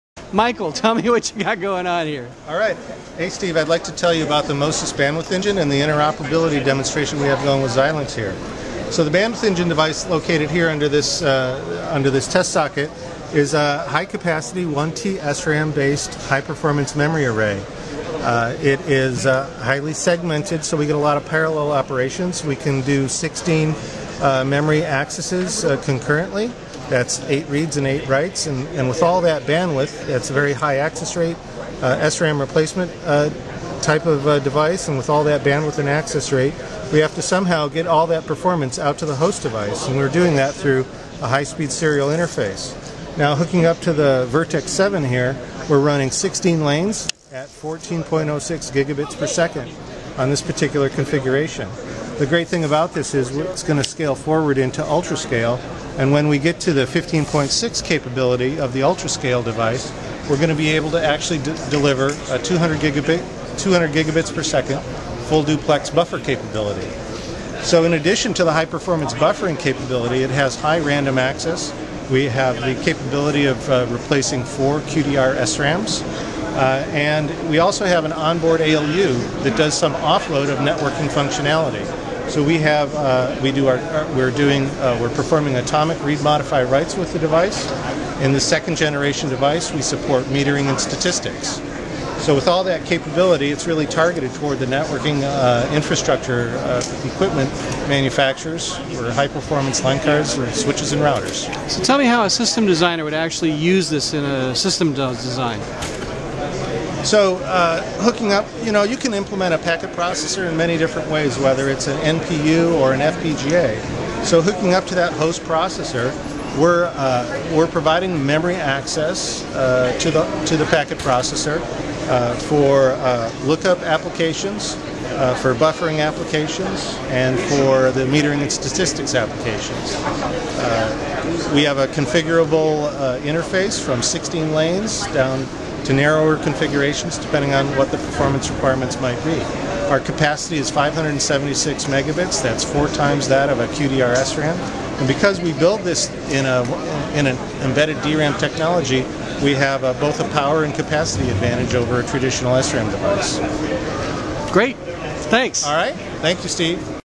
Mosys DesignCon 2014
Demonstration of MoSys Bandwidth Engine 2 with Xilinx Virtex-7 FPGA at DesignCon 2014 in Santa Clara, CA.